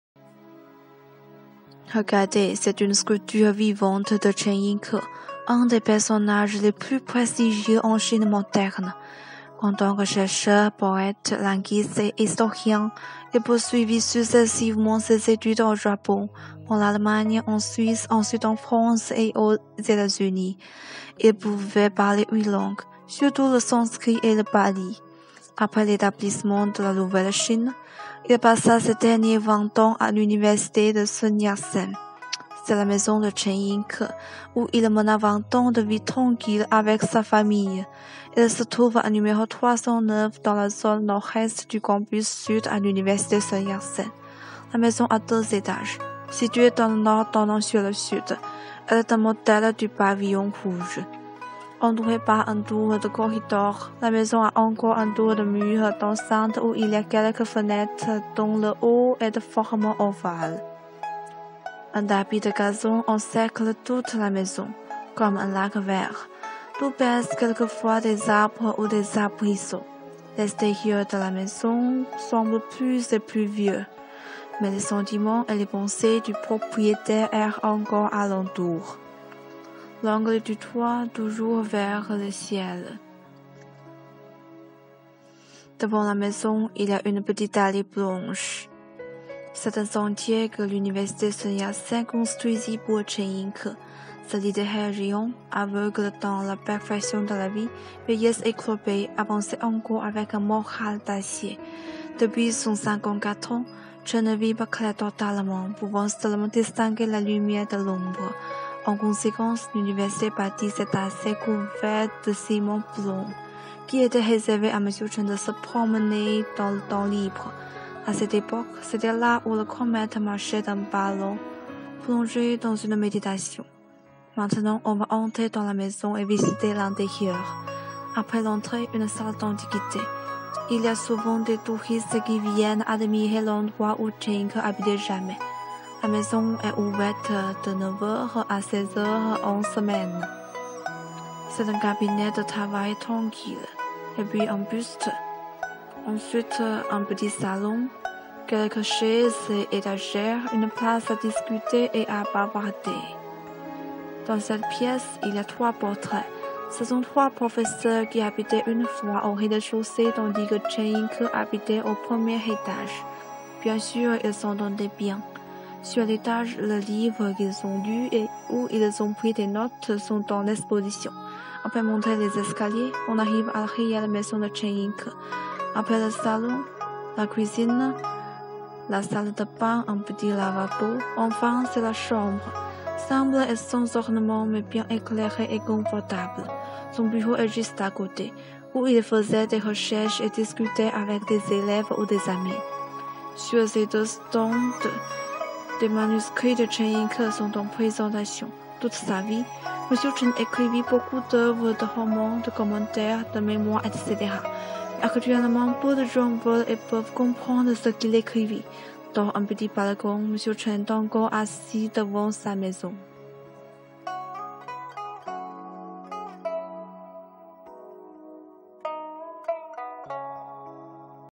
La maison de Chen Yinke
Présentation de la maison de Chen Yinke.

On excusera l'étudiante pour son ton légèrement monotone.

Pistes pédagogiques  :

1) présentez des "maisons" d'écrivains français célèbres : Rousseau (Chambéry, les Charmettes) George Sand...
2) La nouvelle Chine ? qu'est-ce que cela signifie ?